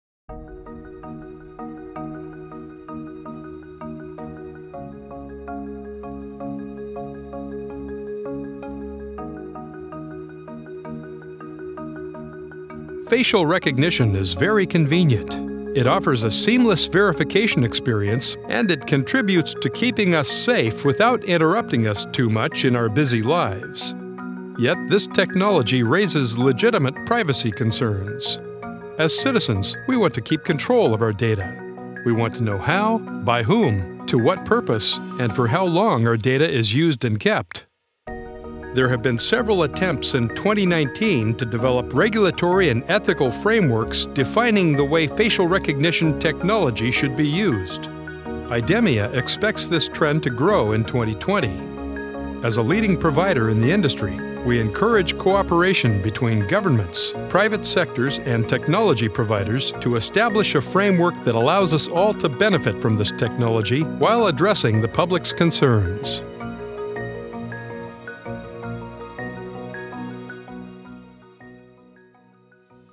Trend 4 - Development of a regulatory and ethical framework for the use of facial recognition
Today, facial recognition technology offers significant problem-solving potential for both security- and convenience-related use cases. Yet as its use is based on monitoring people’s movements, this specific type of biometric data is particularly personal. Why keeping control of our biometric data is so crucial?